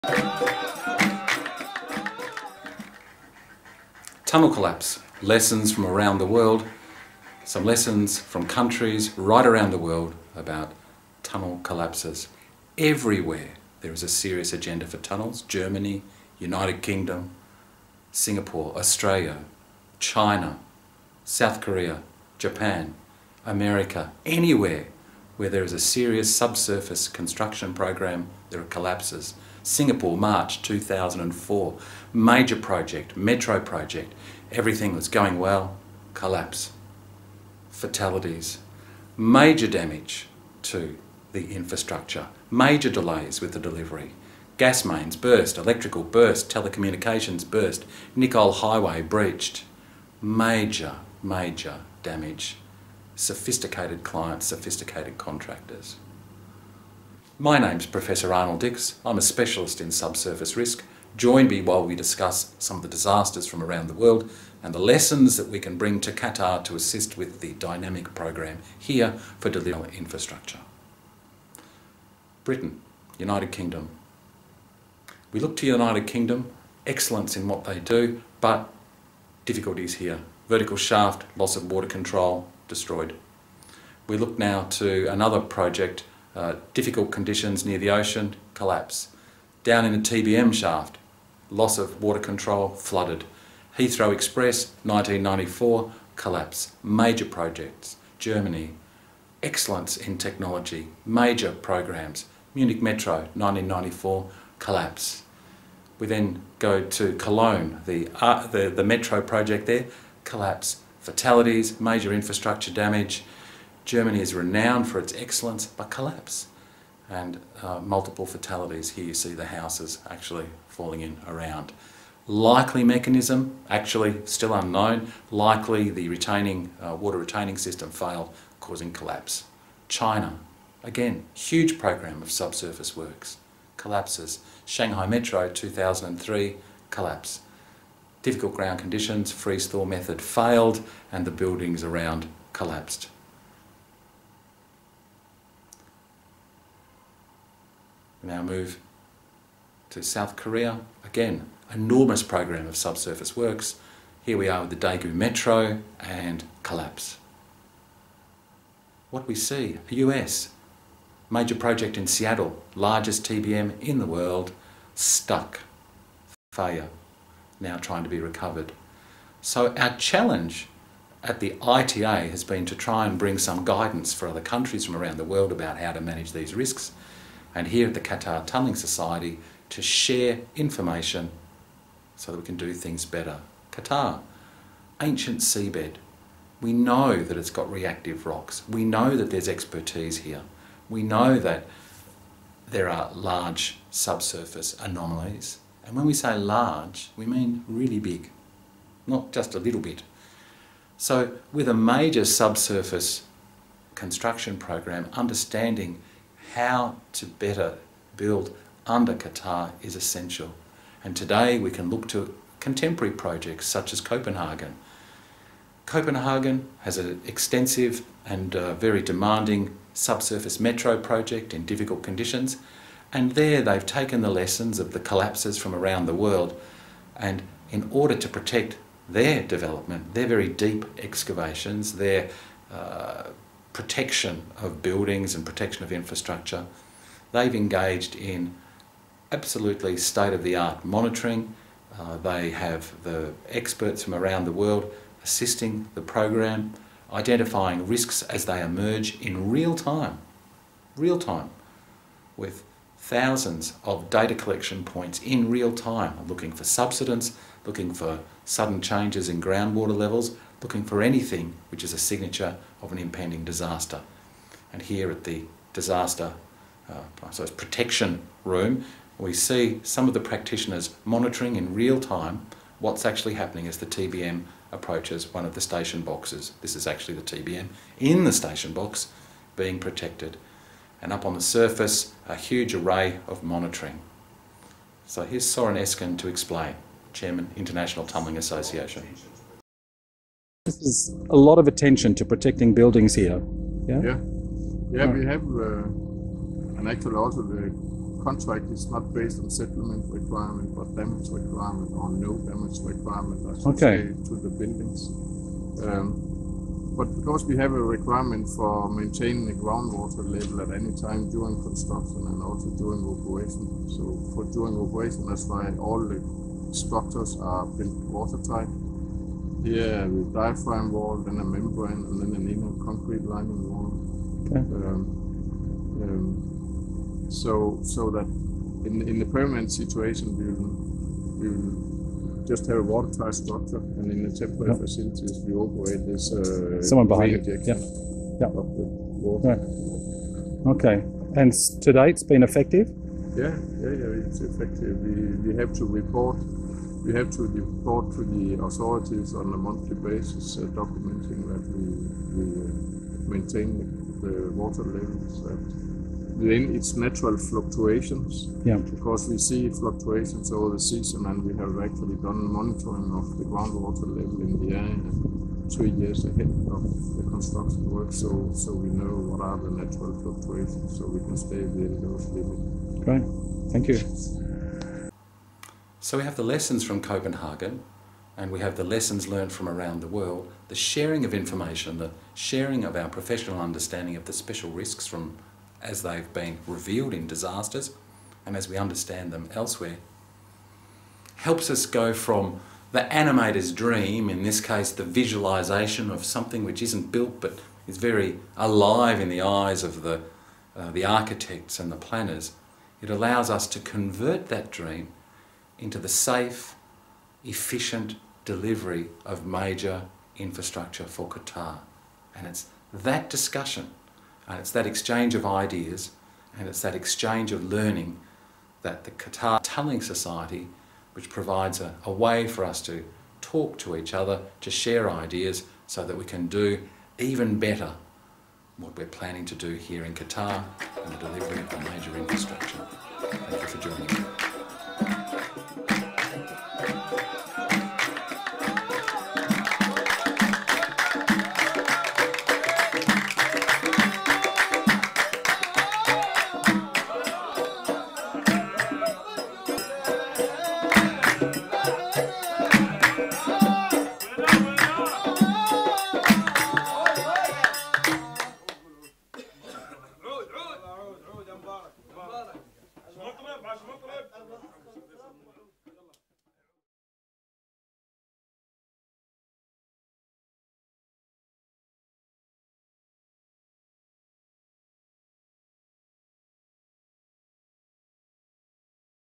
World Tunnel Collapses December 2014 - Canceled presentation Qatar
This presentation was prepared for QSE in late 2014  but was canceled due to lack of interest.